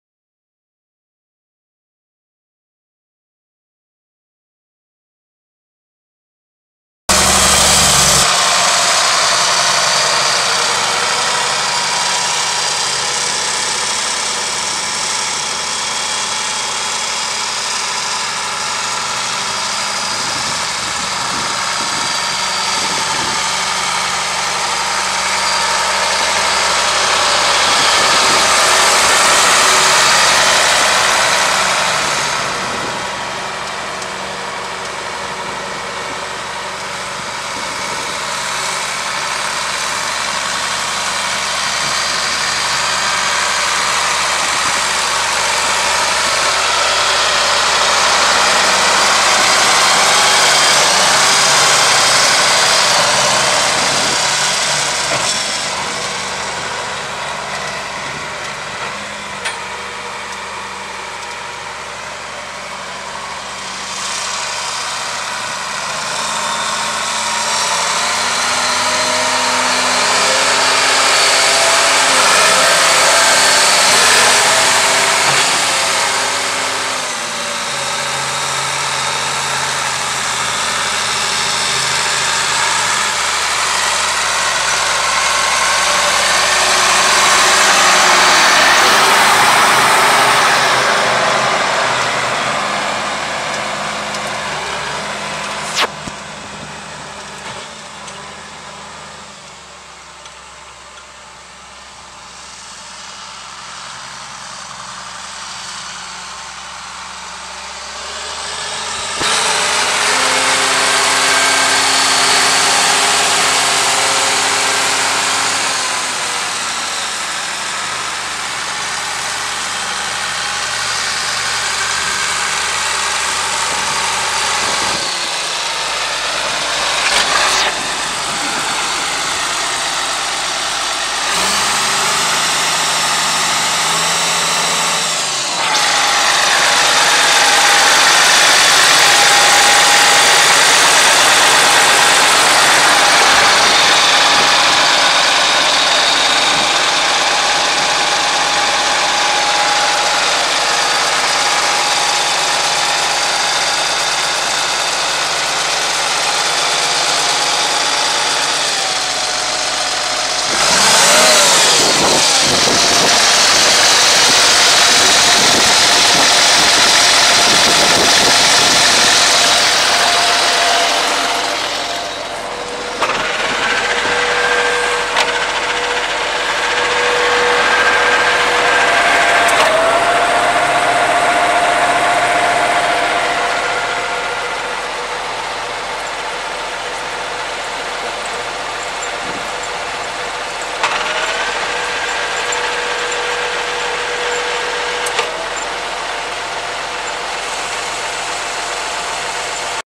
1976 Case 1570 Spirit of 76 tractor for sale | sold at auction September 24, 2014
1976 Case 1570 Spirit of 76 tractor
2,757 hours on meter
8,974 total hours
Case six cylinder engine
152 HP
Engine rebuilt at 6,217 hours
Serial 10007408
Powershift transmission
12F-4R gears
AC and heat
AM/FM/cassette
ROPS serial 7856467
Three point
Draw bar
1000 PTO
(2) rear hydraulic remotes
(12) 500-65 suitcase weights
Wheel weights
11.00-16SL front tires
18.4R38 rear tires
20,500 lbs. base operating weight
Three point has intermittent issues of going up and down
Left rear brake sticks intermittently
Has been repainted and is different then original paint scheme that was originally on the tractor
Serial 8801793
View repairs

It was located in Hesston, KS, and we sold it to the highest bidder for $6,710.